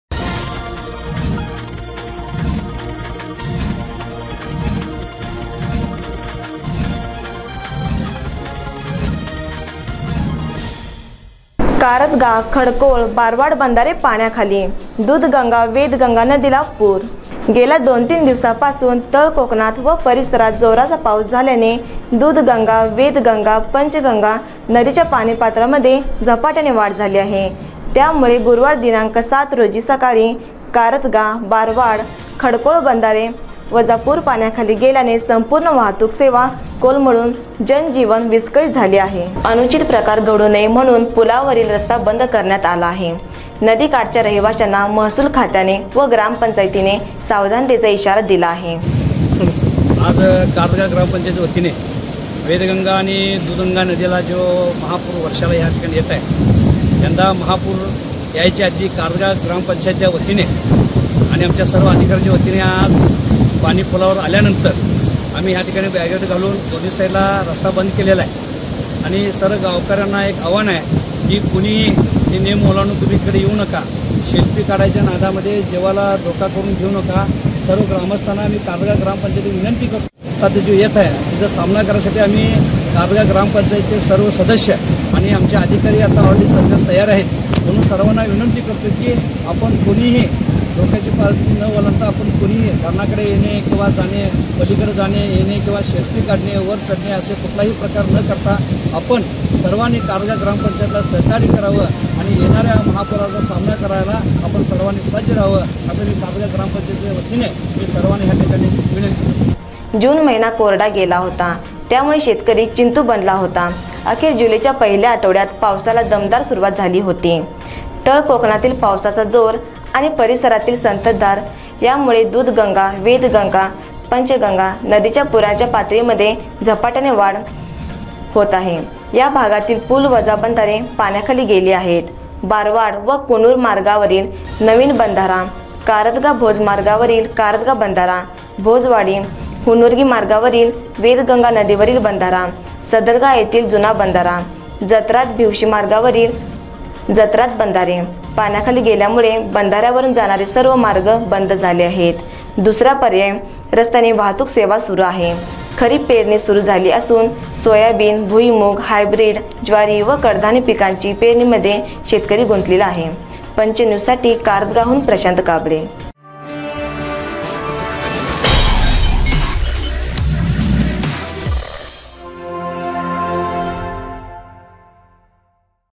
कारदगा ,खडकोळ, बारवाड बंधारे पाण्याखाली..! दुधगंगा वेदगंगा नदीला पूर ..!!
गेल्या दोन-तीन दिवसापासून तळकोकणात व परिसरात जोराचा पाऊस झाल्याने दूधगंगा, वेदगंगा, पंचगंगा ,नदीच्या पाणी पात्रात झपाट्याने वाढ झाली आहे. त्यामुळे गुरूवार दि.०७ रोजी सकाळी कारदगा, बारवाड, खडकोळ बंधारे वजा पूल पाण्याखाली गेल्याने संपूर्ण वहातुक सेवा कोलमडून जनजीवन विस्कळीत झाले आहे.